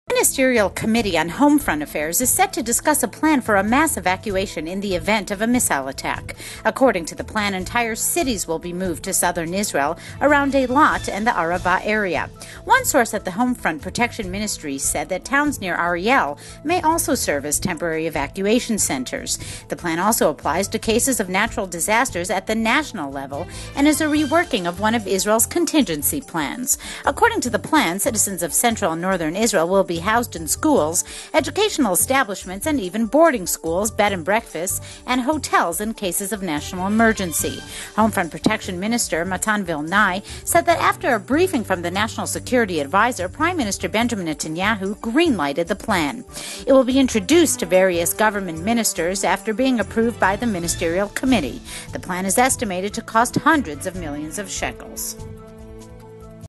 Israel's mass evacuation plan
The Ministerial Committee on Home Front Affairs is set to discuss a
plan for a mass evacuation in the event of a missile attack. According
to the plan, entire cities will be moved to southern Israel around
Eilat and the Arava area. One source at the Home Front Protection
Ministry said that towns near Ariel may also serve as temporary
evacuation centers. The plan also applies to cases of natural
disasters at the national level and is a reworking of one of Israel's
contingency plans. According to the plan, citizens of central and
northern Israel will be housed in schools, educational establishments
and even boarding schools, bed and breakfasts and hotels in cases of
national emergency. Home Front Protection Minister Matan Vilnai said
that after a briefing from the national security adviser, Prime
Minister Benjamin Netanyahu green-lighted the plan. It will be
introduced to various government ministers after being approved by the
ministerial committee. The plan is estimated to cost hundreds of
millions of shekels.